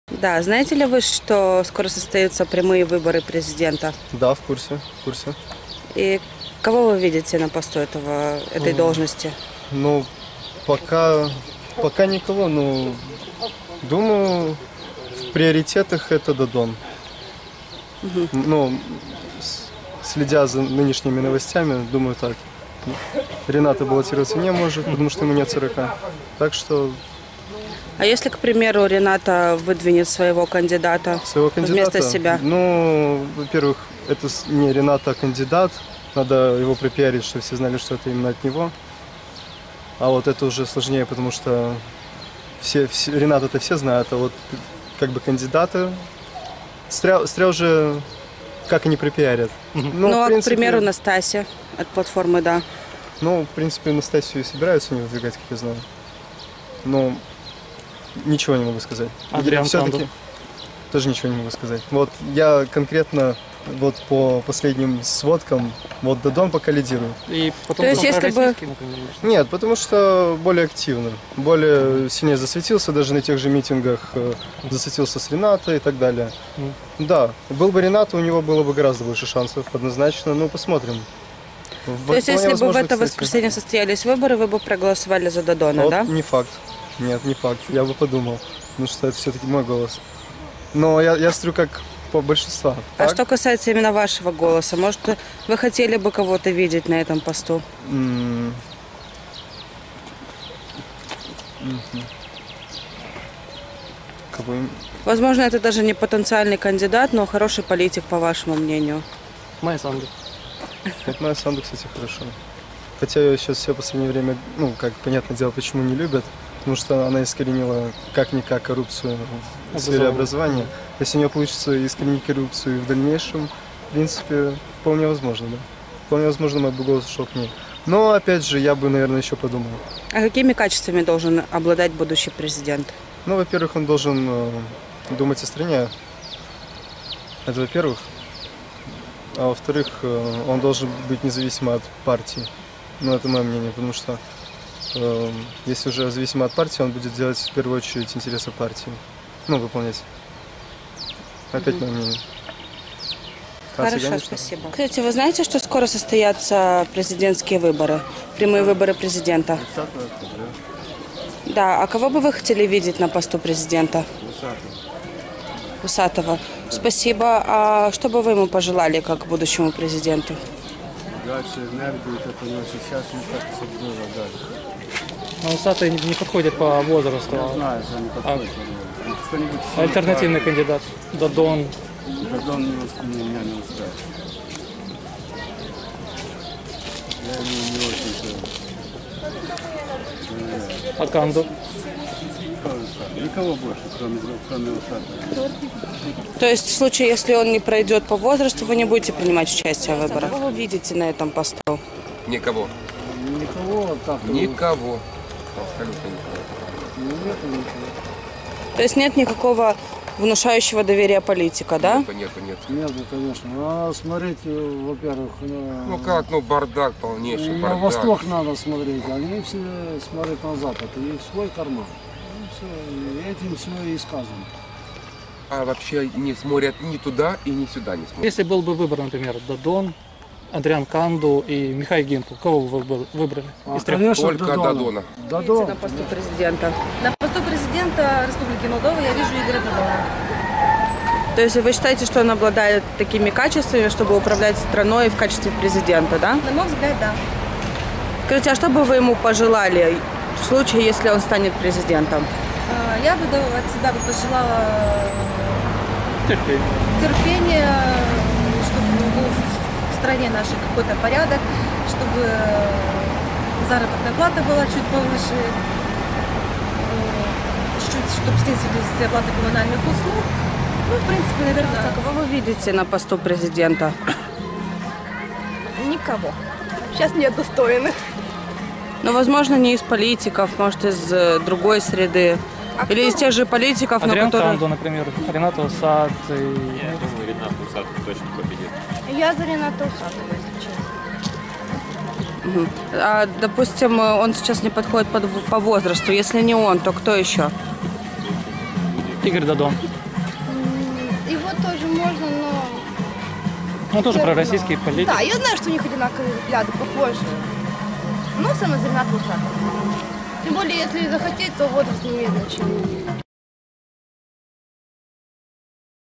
Опрос: кого бельчане видят на посту президента Молдовы? 02.06.2016
Не успел Конституционный суд внести изменения в Закон о выборах Президента в марте 2016 года, как последовала предвыборная гонка. Опрос на улицах города позволил выявить фаворита.